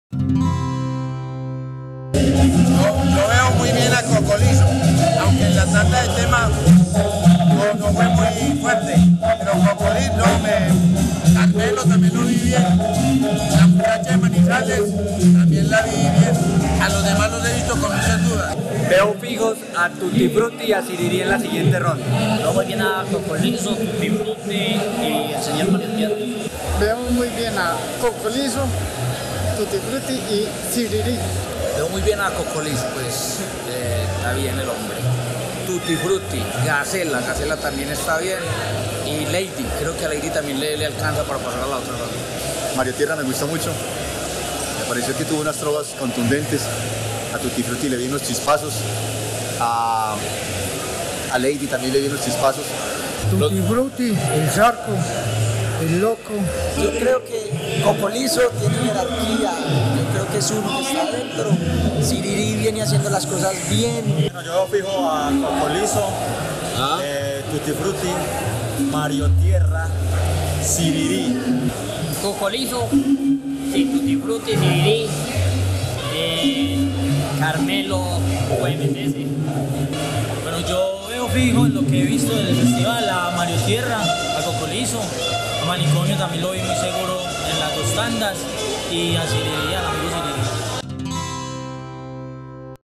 El veredicto del público. Primera ronda, segunda clasificatoria. Festival Ciudad de Medellín
la opinión del público sobre los trovadores que debieron haber pasado a la segunda ronda de la segunda clasificatoria del Festival de la trova, Ciudad de Medellín.